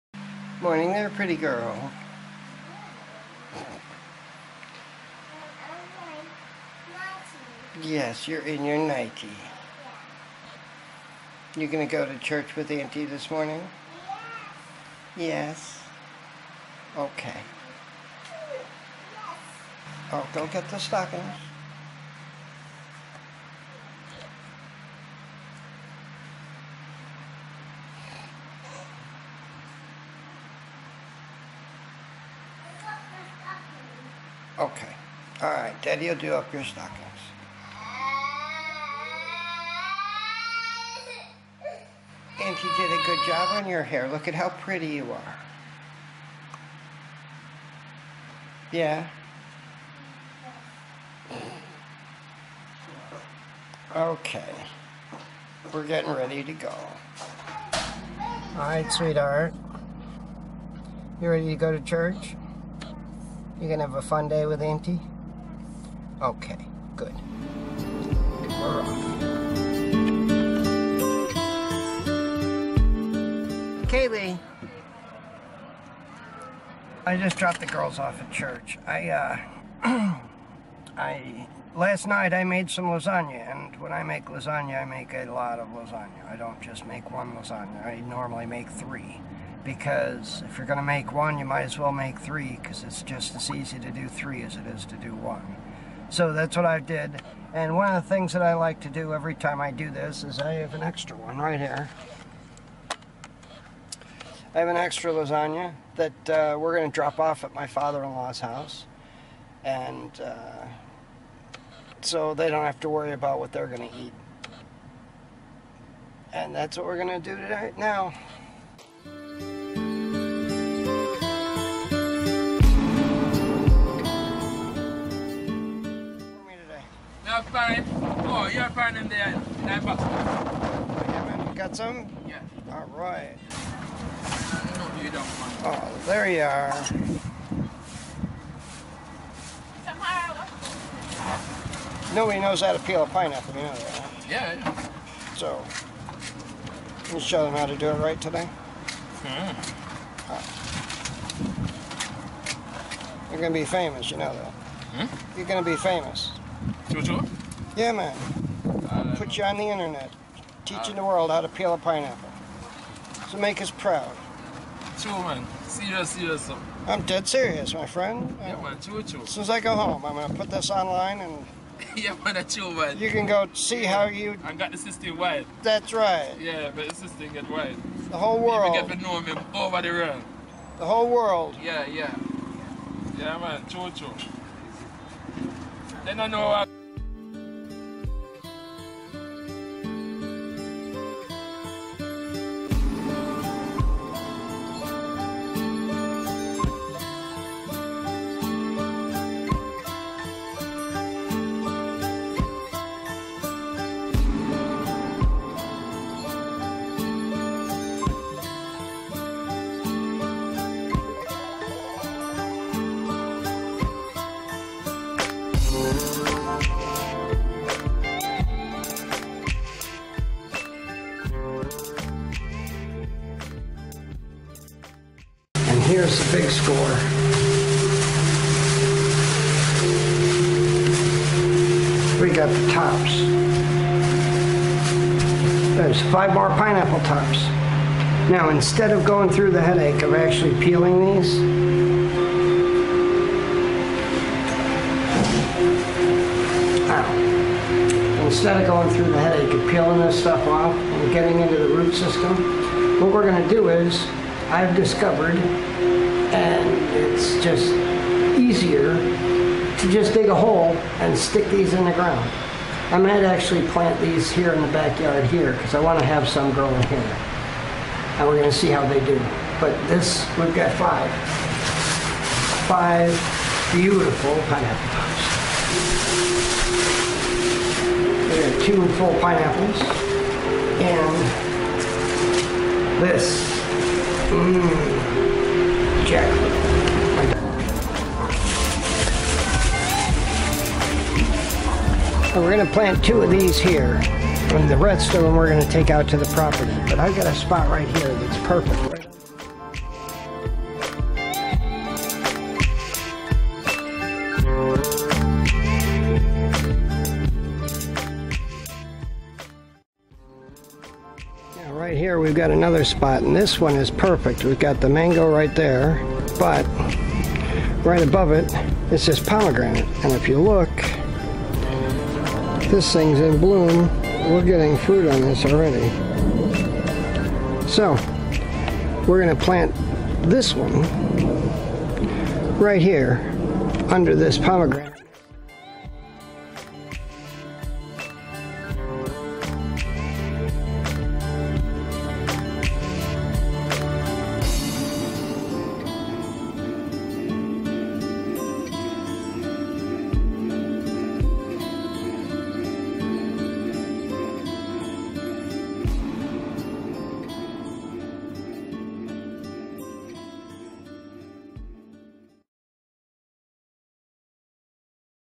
How To Peel a Pineapple
Had a relaxing day today. Took the girls to church, dropped off a lasagna at my father in laws house and stopped by the fruit stand.  The guy at the fruit stand shows the proper way to peel a pineapple. I get lucky too, he had a few extra pineapple tops for me along with a really good jack fruit. Even got 2 of the pineapple tops planted.